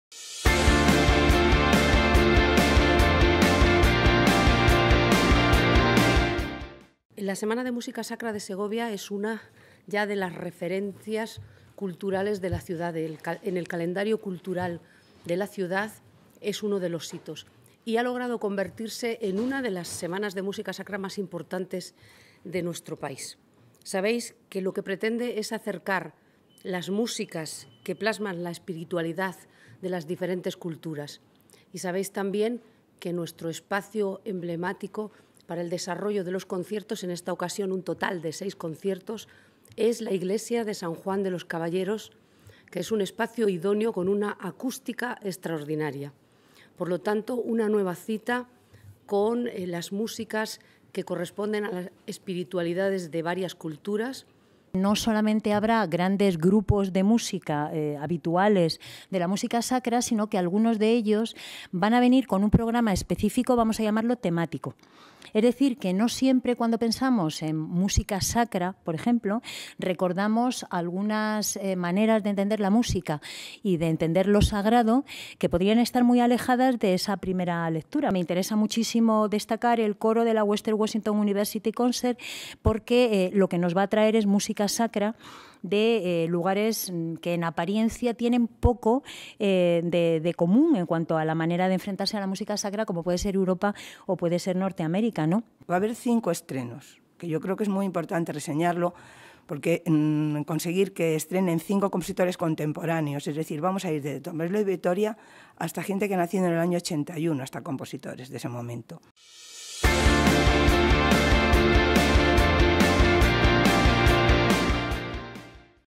El Festival de Música Sacra volverá a sonar en San Juan de los Caballeros del 19 al 24
La '34 Semana de Música Sacra de Segovia' propone un programa en el que, a los sonidos más tradicionales del género, se unirán los sonidos gospel o la percusión.